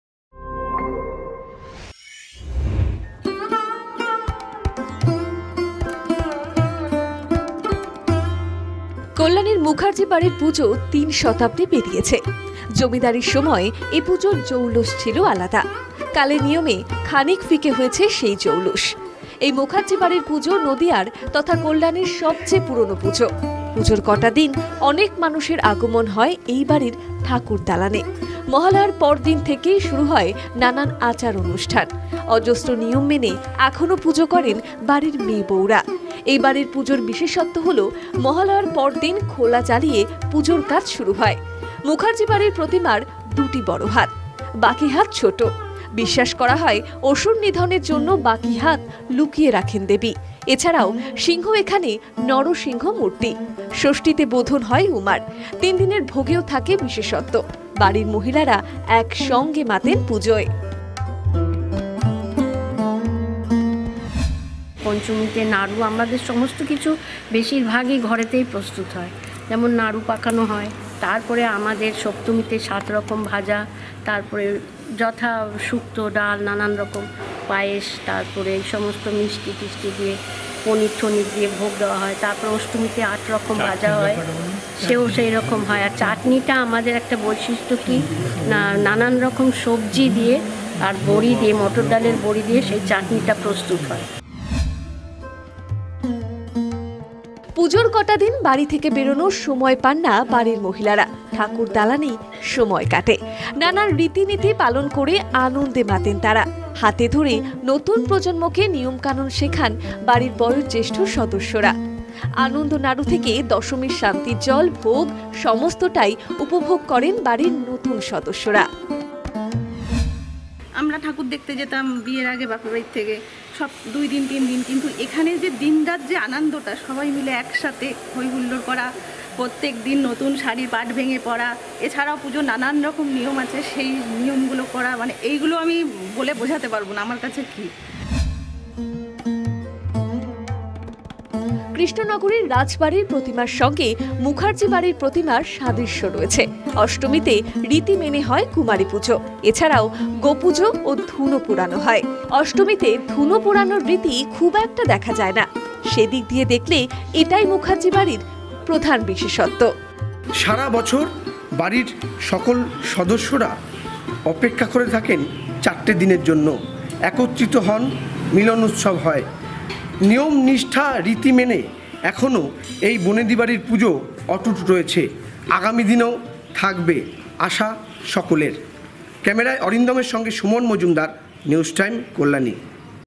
সাড়ে তিনশো বছরের পুরনো মুখার্জী বাড়ির পুজো । News Time Bangla
কল্যানীর প্রাচীন পুজো মুখার্জী বাড়ির পুজো। সময়ের ভারে বদলেছে অনেক কিছুই, জমিদারির দিন গিয়েছে। প্রভাব প্রতিপতির জাঁকজমক খানিক স্নান হয়েছে। তবে প্রতিবছর পুজো হয় নিয়ম মেনে। ঠাকুরদালানে প্রতিমা নির্মাণ শুরু হলেই পরিবারের সদস্যরা দিন গোনেন, কবে উমা আসবেন। ওই চারটে দিন যেন পরিবারের মিলন উৎসব।